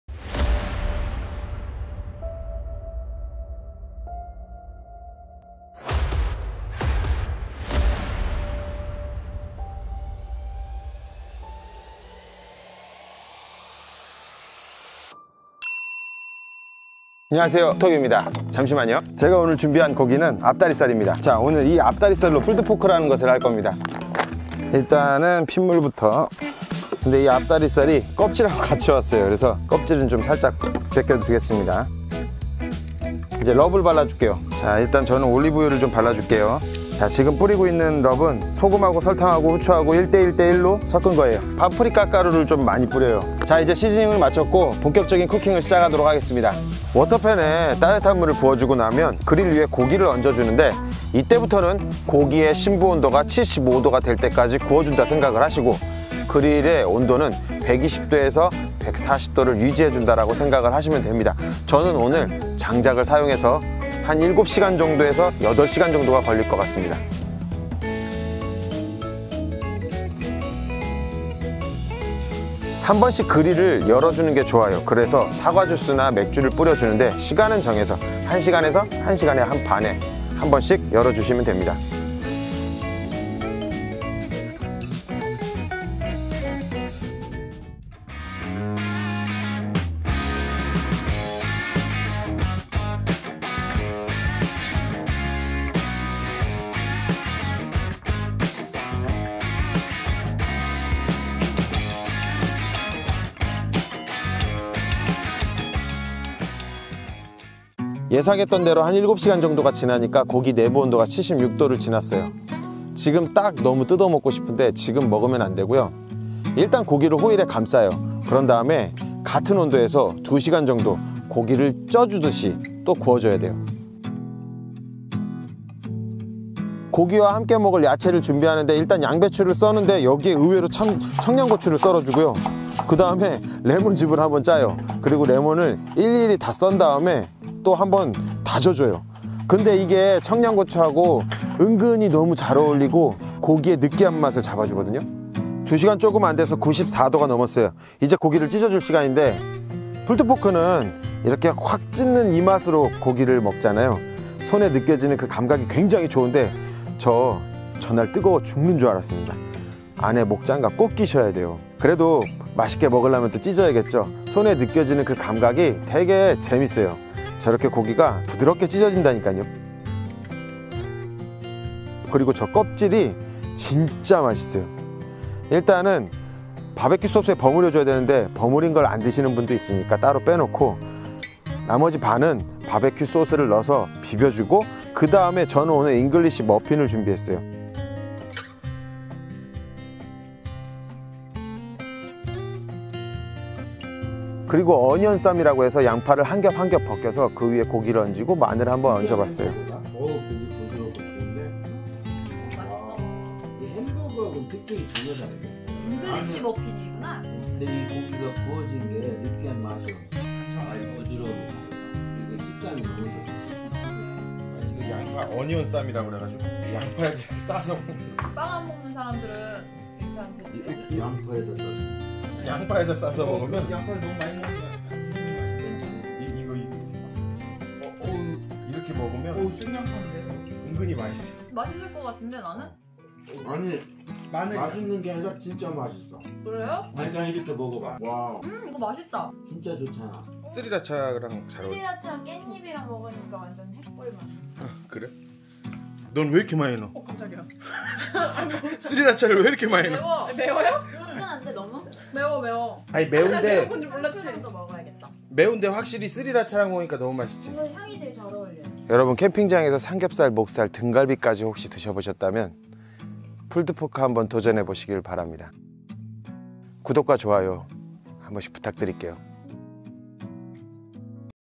풀드포크 - 손으로 찢는 그맛에 먹는 바베큐 / 텍사스 전통 바베큐!!
안녕하세요^^
오늘은 드디어 텍사스 바베큐를 했습니다.
풀드포크~ 풀드포크 다들 이야기 하시지만 그릴에 하는 풀드포크는 많지
않기에 시도한 영상입니다.
그중에서도 가장 쉽고 실패가 없다는 풀드포크를 했습니다.
그런데 쉽지 않아 고생 좀 했습니다.
예전에 실패한 적도 있고요.
그런데 이제 실패하지 않습니다.
오늘은 목살이 아닌 앞다리살로 풀드포크를 진행했습니다.
재밌게 봐주시고 앞으로도 그릴에 할 수 있는 바베큐를 열심히 도전해 보겠습니다.
감사합니다.
자주봐요 우리!!!

구독~!